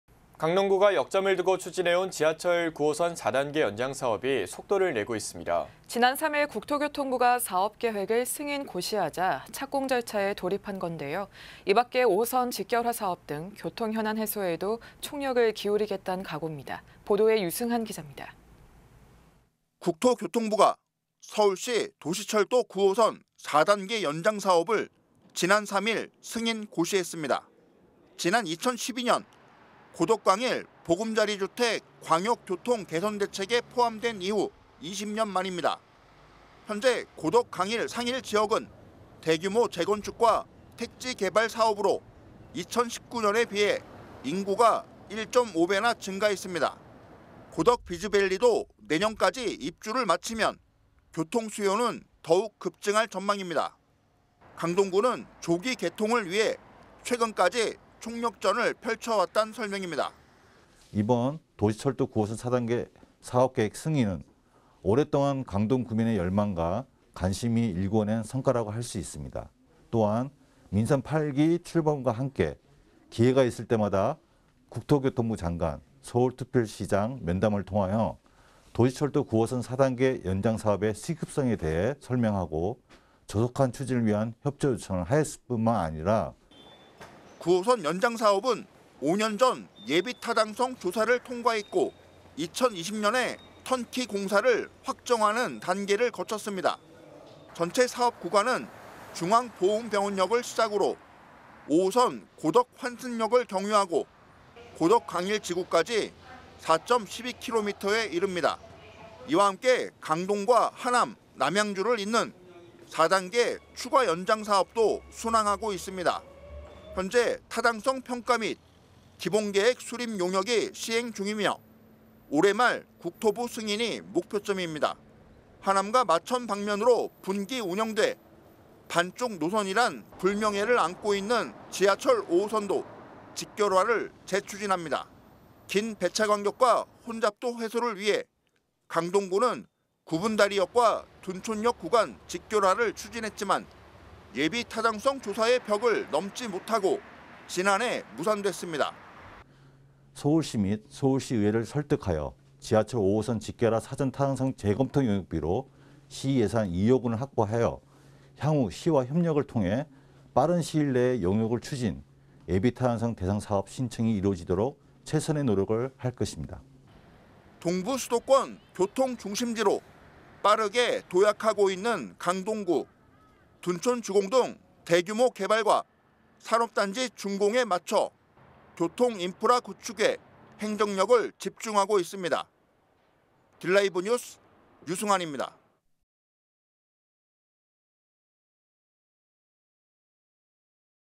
[강동] 강동구, 동부수도권 교통 중심지 '박차'
진행 : 박세종 아나운서, 민세정 아나운서

【 앵커멘트 】
강동구가
역점을 두고 추진해온
지하철 9호선 4단계 연장사업이
속도를 내고 있습니다.
지난 3일 국토교통부가
사업계획을 승인, 고시하자
착공 절차에 돌입한 건데요.
이 밖에 5호선 직결화 사업 등
교통 현안 해소에도 총력을
기울이겠단 각옵니다.
보도에 유승한 기잡니다.

【 리포트 】
국토교통부가
서울도시철도 9호선 4단계 연장사업을
지난 3일 승인 고시했습니다.

지난 2012년 고덕강일 보금자리주택
광역교통개선대책에 포함된 이후
20년 만입니다.

현재 고덕, 강일, 상일지역은
대규모 재건축과 택지개발사업으로
2019년에 비해 인구가 1.5배나 증가했습니다.

고덕비즈밸리도 내년까지 입주를 마치면
교통수요는 더욱 급증할 전망입니다.

강동구는 조기 개통을 위해
최근까지 총력전을 펼쳐왔단 설명입니다.

( 최재민 팀장 / 강동구청 교통행정과 )
"이번 도시철도 9호선 4단계
사업계획 승인은 오랫동안
강동구민의 열망과 관심이 일구어낸
성과라고 할 수 있으며 민선8기 출범과 함께
기회가 있을 때마다 국토교통부 장관,
서울시장 면담을 통해 도시철도 9호선 4단계
연장사업의 시급성에 대해 설명하고
조속한 추진을 위한 협조를 요청했을 뿐만 아니라…"

9호선 연장사업은
5년 전 예비타당성 조사를 통과했고
2020년에 턴키공사를 확정하는 단계를
거쳤습니다.

전체 사업구간은
중앙보훈병원역을 시작으로
5호선 고덕 환승역을 경유하고
고덕강일지구까지 4.12km에 이릅니다.

이와 함께 강동과 하남 남양주를 잇는
4단계 추가 연장 사업도 순항하고 있습니다.

현재 타당성 평가 및 기본계획 수립 용역이
시행 중이며 올해 말 국토부 승인이
목표점입니다.

하남과 마천 방면으로 분기 운영돼
반쪽 노선이란 불명예를 안고 있는
지하철 5호선도 직결화를 재추진합니다.

긴 배차간격과 혼잡도 해소를 위해
강동구는 굽은다리역과 둔촌역 구간
직결화를 추진했지만 예비타당성조사의
벽을 넘지 못하고 지난해 무산됐습니다.

"서울시 및 서울시의회를 설득해
지하철5호선 직결화 사전타당성
재검토 용역비로 시 예산 2억 원을
확보하여 향후 시와 협력을 통해
빠른 시일 내에 용역을 추진
예비타당성 대상사업 신청이
이루어지도록 최선의 노력을 할 것입니다."

동부 수도권 교통 중심지로
빠르게 도약하고 있는 강동구.

둔촌주공 등 대규모 개발과
산업단지 준공에 맞춰 교통인프라
구축에 행정력을 집중하고 있습니다.

딜라이브뉴스 유승한입니다.